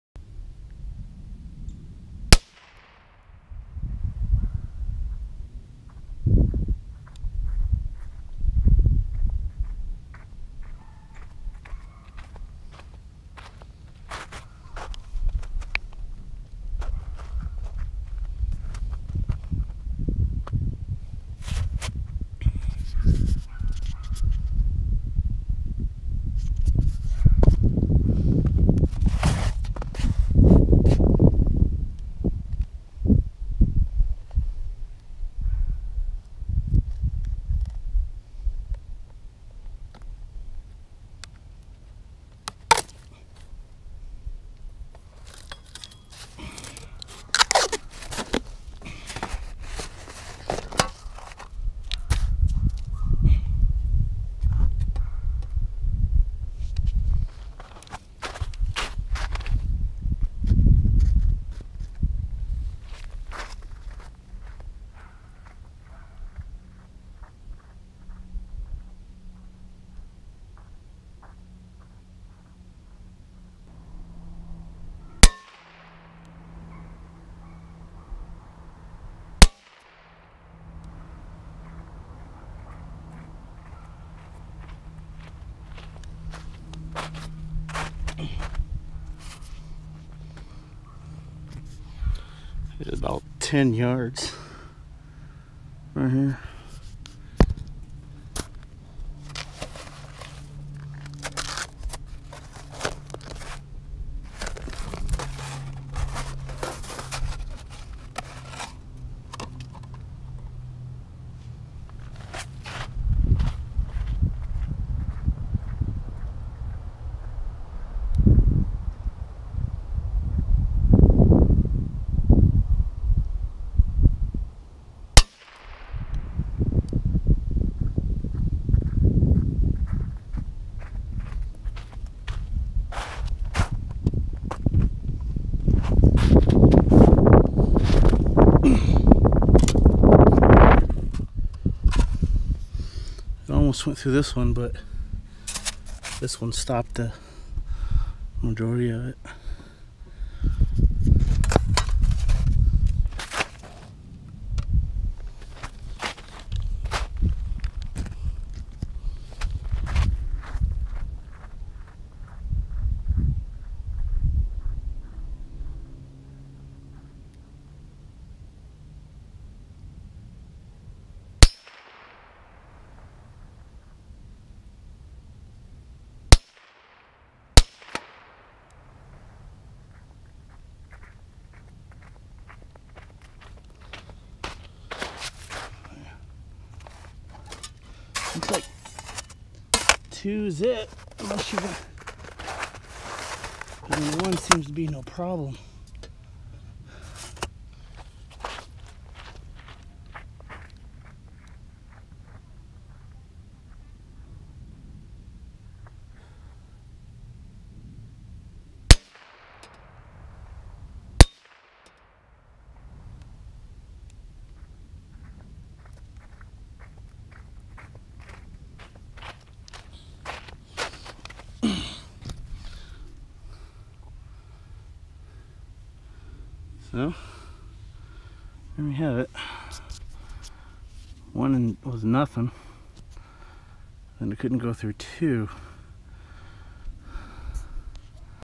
Shooting Dividers
How many dividers will a .22 round go through? I will probably try this again, but will use other types of ammo.